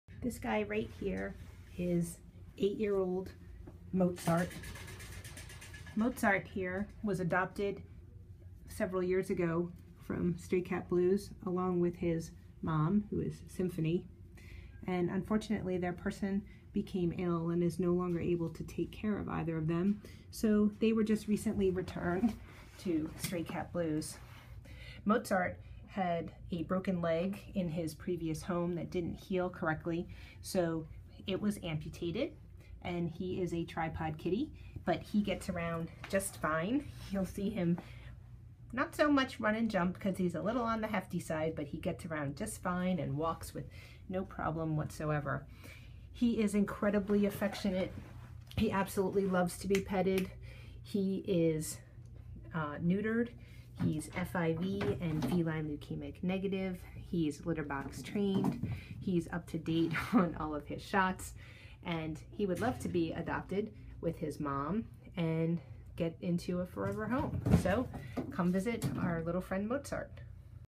Mozart
Mozart's Info...
I am already neutered and up to date with shots.

Mozart's Story...
Mozart (shown here) and Symphony
Symphony is a sweet kitty! And Mozart, her son, is a momma’s boy and a love!

This mother and son have been together for seven years. They were originally rescued along with five more male kittens from Pottstown, all of which were adopted. Mozart and Symphony were adopted together. Sadly, their person is very ill and can no longer care for them, so they were returned to the Stray Cat Blues network. They would love to be able to stay together in their new home.

They are both healthy, fixed, have up to date vaccinations, are microchipped, and have been bathed and groomed. Motz had to have his right rear leg amputated due to a non-healing brake. And he should probably lose a few pounds!

Symphony is estimated to be 13 years old, and Mozart is 7 years old.

Like what you see? to access the Stray Cat Blues page, where you can see information on our adoption process and access a link to our online adoption application. And Thank You for considering adopting this cat